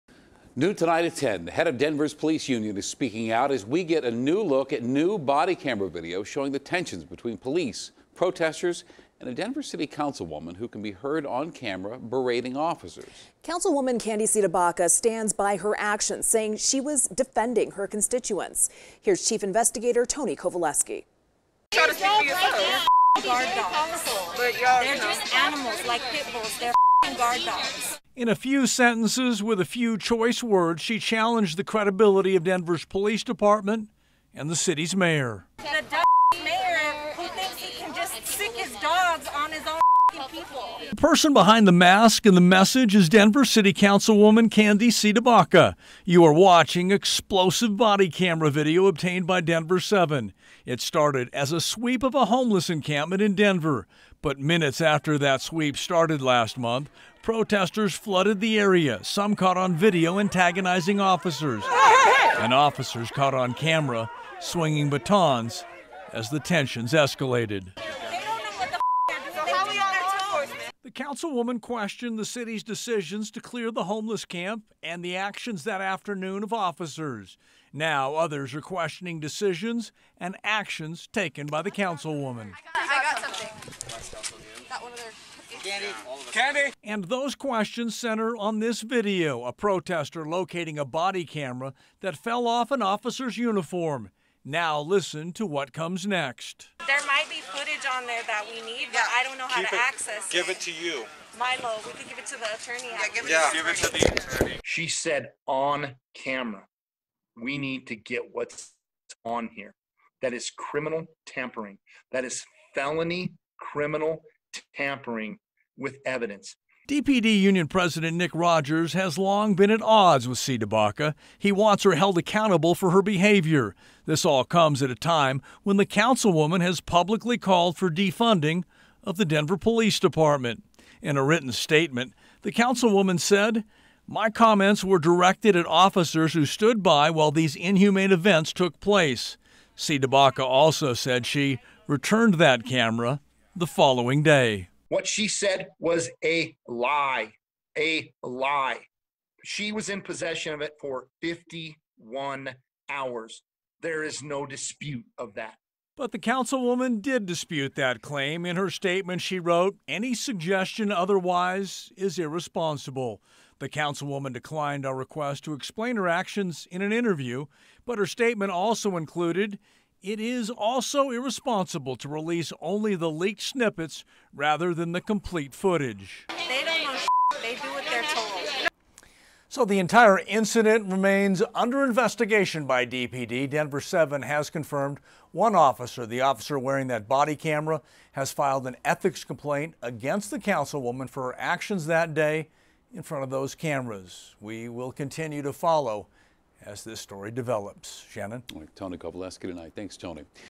Councilwoman CdeBaca discusses police-protester confrontation after more body camera released
Through an open records request, Denver7 has received copies of police body camera video showing more of what took place during a confrontation between police and a group of people protesting a homeless cleanup on Aug. 19.